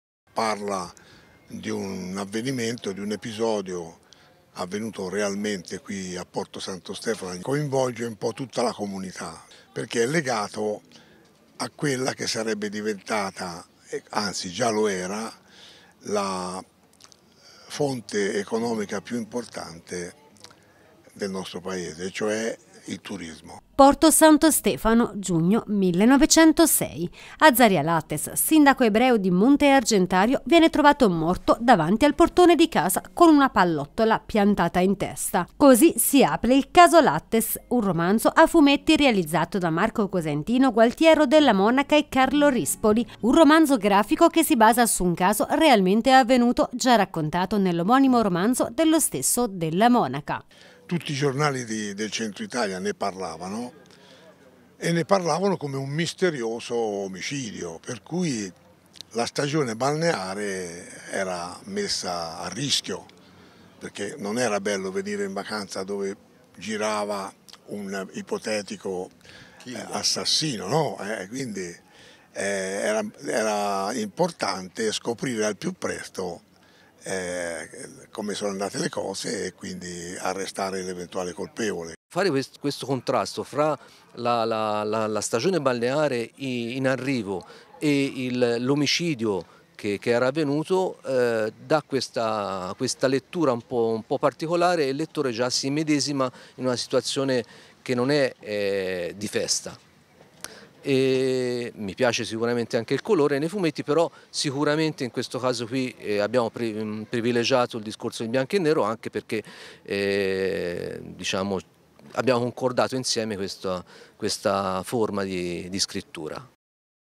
Il "Caso Lattes" diventa un romanzo a fumetti
Santo Stefano, giugno 1906. Azzaria Lattes, sindaco ebreo di Monte Argentario, viene trovato morto davanti al portone di casa, con una pallottola piantata in testa.
Così si apre "Il caso Lattes", un romanzo a fumetti realizzato da Marco Consentino, Gualtiero Della Monaca e Carlo Rispoli.
Un racconto grafico che si basa su un caso realmente avvenuto, già raccontato nell'omonimo romanzo dello stesso Della Monaca.